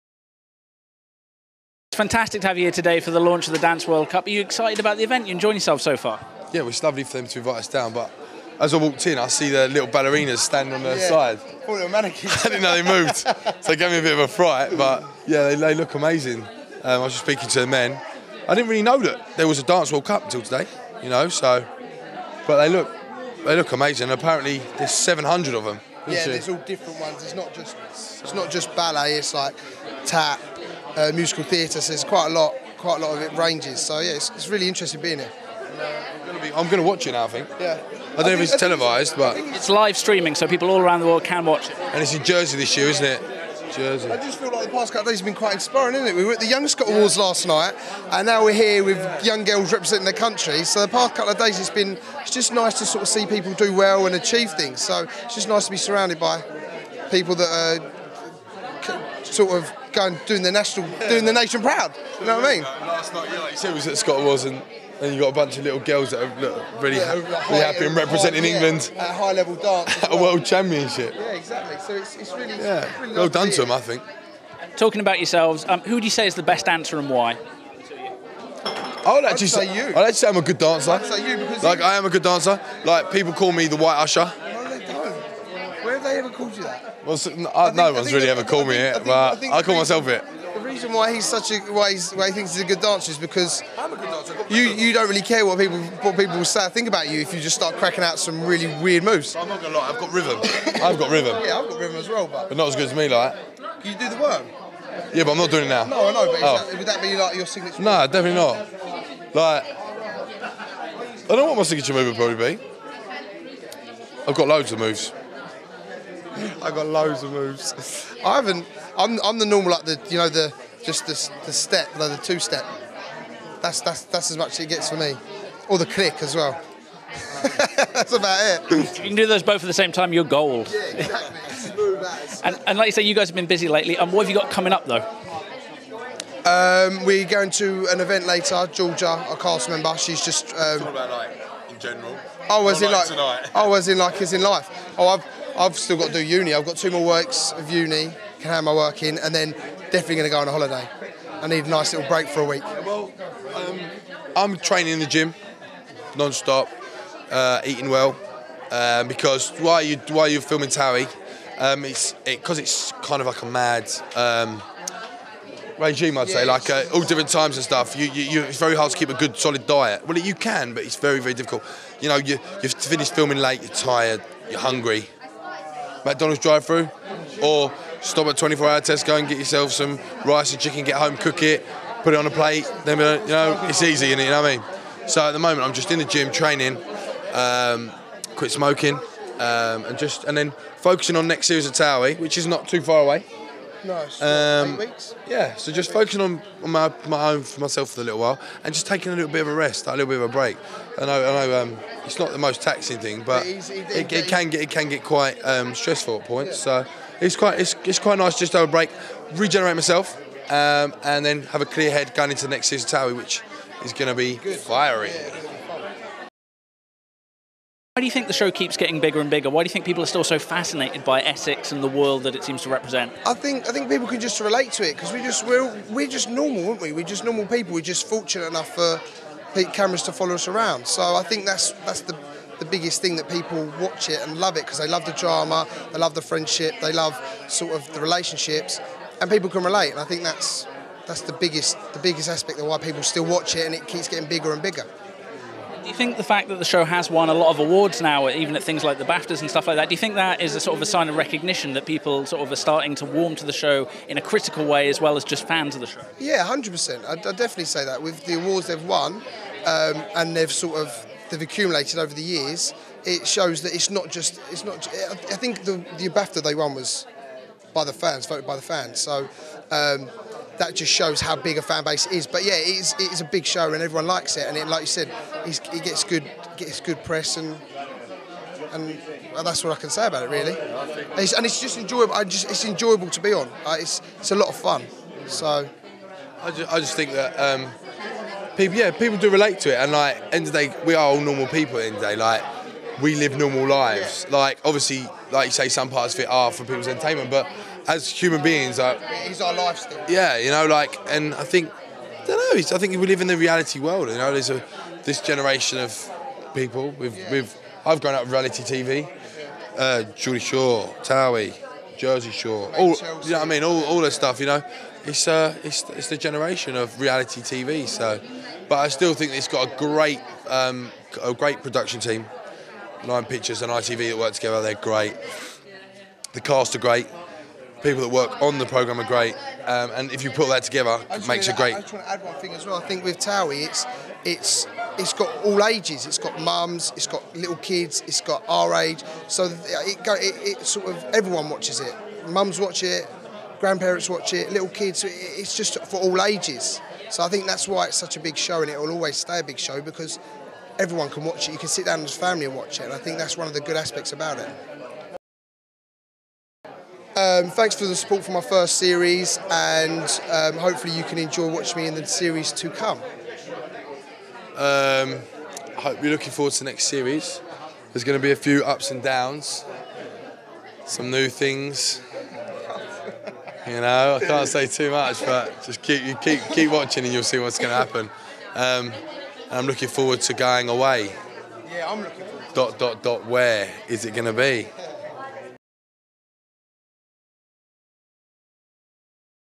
TOWIE Jon & Chris Clark Interview 2016
Red Carpet News TV talks to Kimberly Wyatt, Ella Jade, Pascal Craymer, Bonnie Lythgoe, TOWIE stars Jon & Chris Clark and competition organizers at the London launch event of Dance World Cup 2016